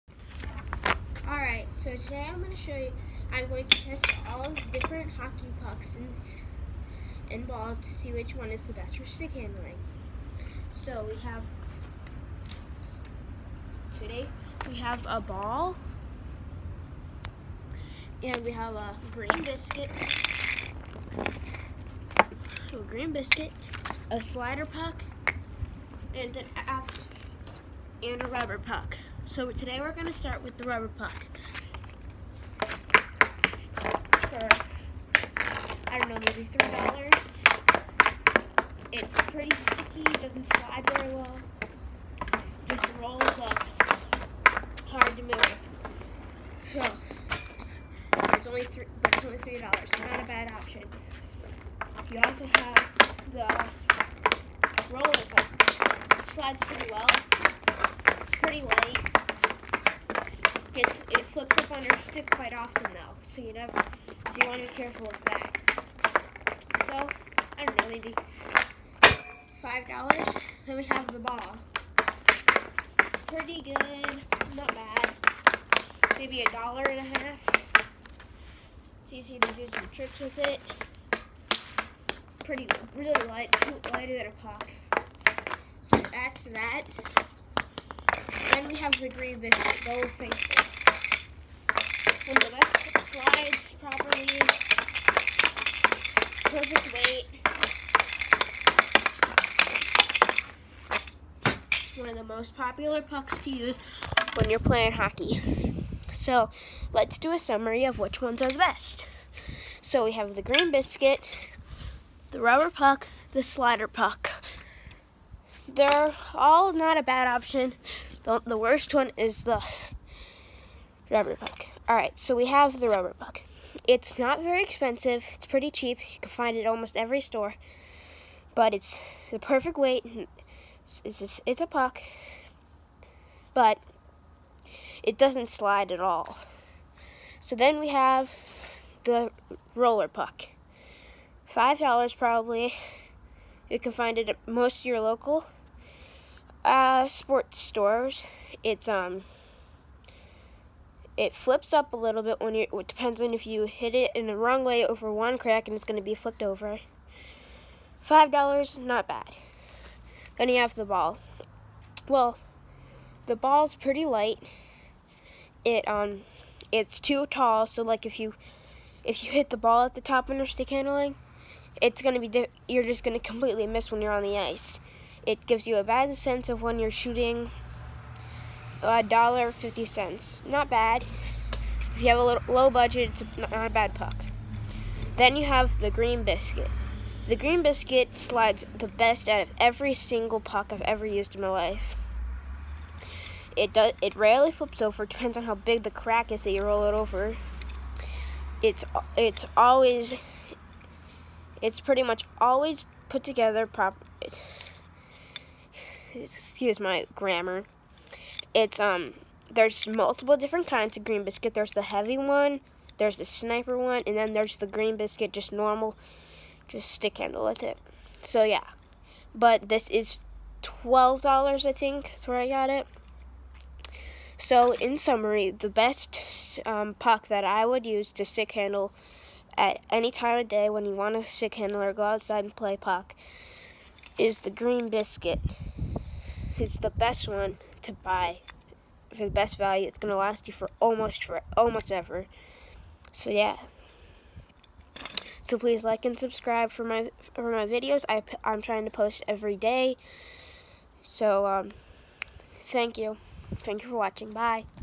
What is the best outdoor hockey puck?
This video goes through the green biscuit vs. slider puck vs. ball vs. rubber puck!